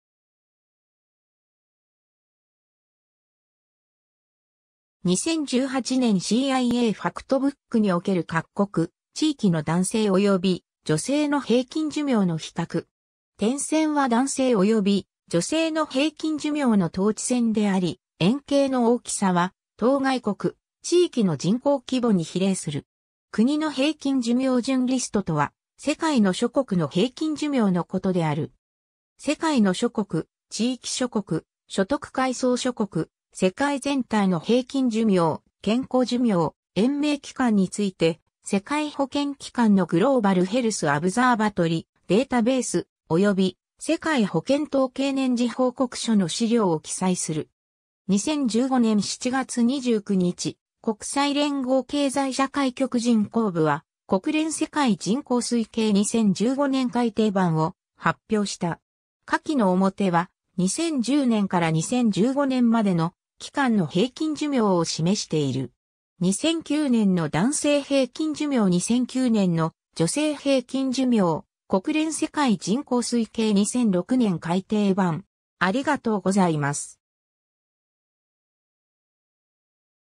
国の平均寿命順リスト
2018年CIAファクトブックにおける各国・地域の男性および女性の平均寿命の比較。
点線は男性および女性の平均寿命の等値線であり、円形の大きさは当該国・地域の人口規模に比例する。
 国の平均寿命順リスト（くにのへいきんじゅみょうじゅんリスト、List of countries by life expectancy）とは、世界の諸国の平均寿命のことである。
世界の諸国・地域諸国・所得階層諸国・世界全体の平均寿命、健康寿命、延命期間について、世界保健機関(World Health Organization)のGlobal Health Observatory (GHO) データベース、および世界保健統計(World Health Statistics)年次報告書の資料を記載する。
 2015年7月29日、国際連合経済社会局 (UN DESA) 人口部は国連世界人口推計2015年改訂版を発表した。
下記の表は、2010年から2015年までの期間の平均寿命を示している。
 2009年の男性平均寿命 2009年の女性平均寿命 国連世界人口推計2006年改訂版 (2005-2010年の平均寿命)